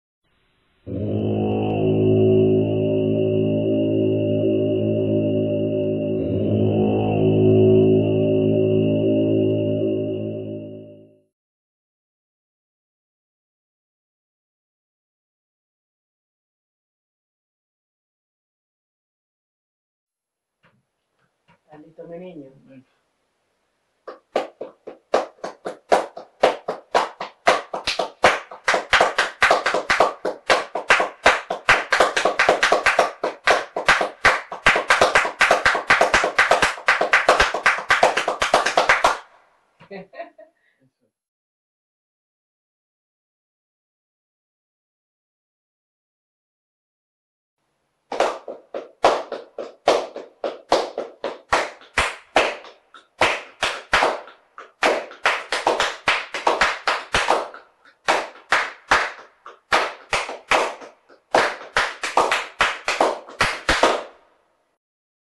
flamenco clapping demo
Flamenco clapping demo for bulerías and sevillanas with off beats